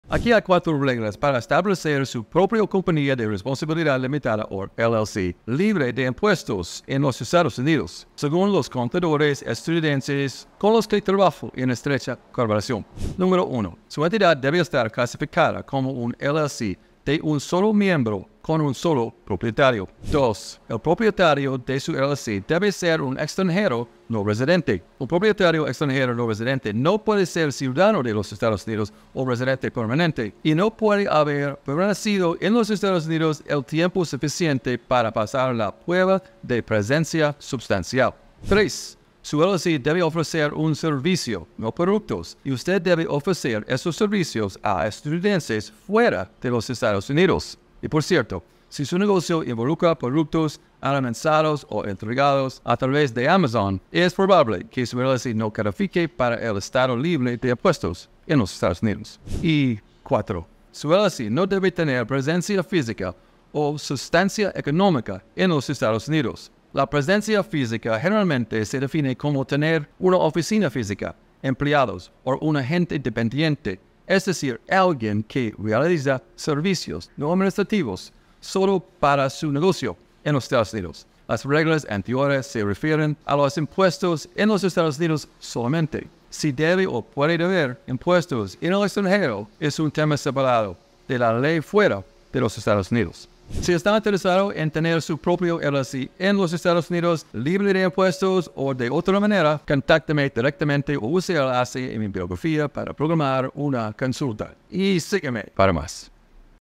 4 reglas para constituir una empresa libre de impuestos en Estados Unidos
Aquí hay 4 reglas para establecer su propia compañía de responsabilidad limitada (o LLC)  libre de impuestos en los Estados Unidos, según los contadores estadounidenses con los que trabajo en estrecha colaboración:

1. Su entidad debe estar clasificada como una LLC de un solo miembro con un solo propietario;

2. El propietario de su LLC debe ser un extranjero no residente

Un propietario extranjero no residente no puede ser ciudadano de los EE. UU. o residente permanente y no puede haber permanecido en los EE. UU. el tiempo suficiente para pasar la "prueba de presencia sustancial".

3. Su LLC debe ofrecer un servicio, no productos, y usted debe ofrecer esos servicios a estadounidenses fuera de los Estados Unidos.

Y, por cierto, si su negocio involucra productos almacenados o entregados a través de Amazon, es probable que su LLC no califique para el estado libre de impuestos en los EE. UU.

4. Su LLC no debe tener presencia física o sustancia económica en los Estados Unidos.

La presencia física generalmente se define como tener una oficina física, empleados o un "agente dependiente" (es decir, alguien que realiza servicios no administrativos solo para su negocio) en los Estados Unidos.

Las reglas anteriores se refieren a los impuestos en los EE. UU. solamente.

Si debe o puede deber impuestos en el extranjero es un tema separado de la ley fuera de los EE. UU.

Si está interesado en tener su propia LLC len los Estados Unidos -  libre de impuestos o de otra manera - contácteme directamente o use el enlace en mi biografía para programar una consulta.

Wherever you go, there we are.